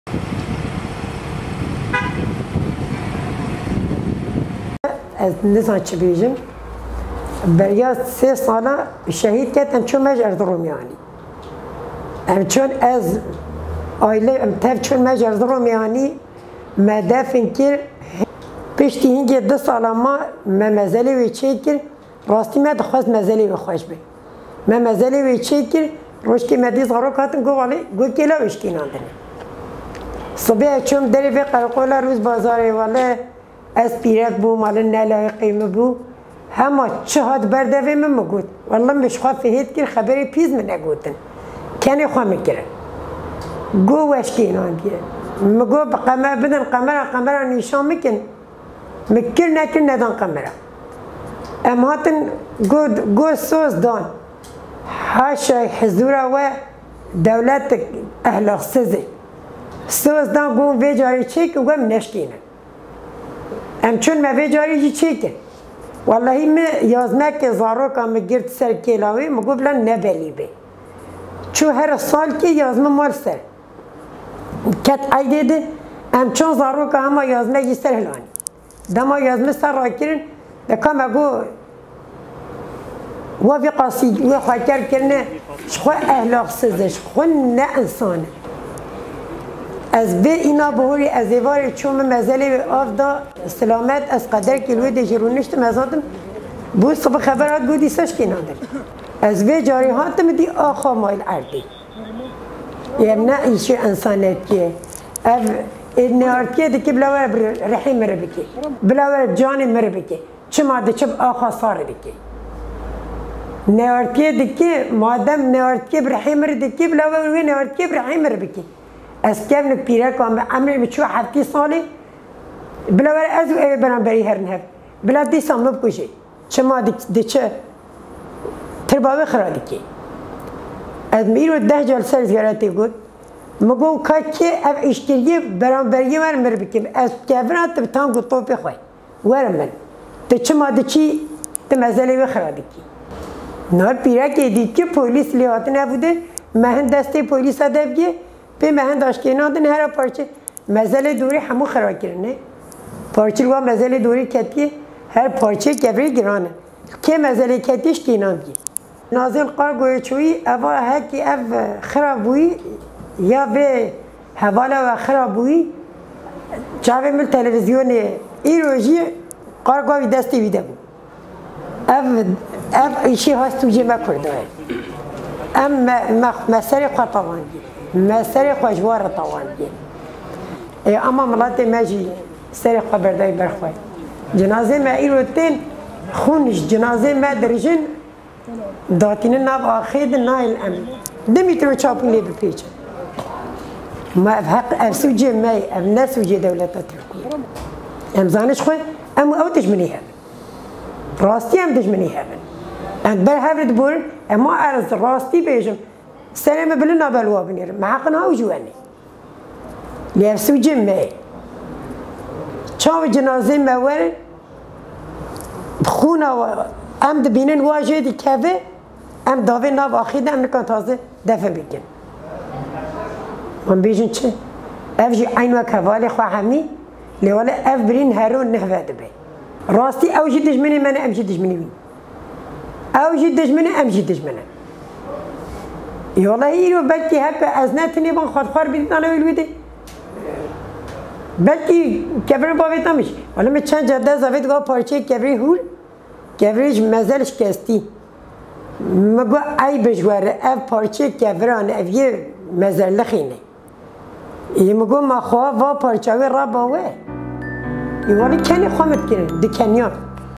AYNI MEZARA İKİNCİ SALDIRI: SESSİZLİK ZULME YOL AÇIYOR
Sêrt’te Revşan Aşkara’nın mezarının bir yıl içinde iki kez tahrip edilmesine tepki gösteren annesi Kudret Aşkara, sessizlik halinin yeni zulümlere yol açtığını söyledi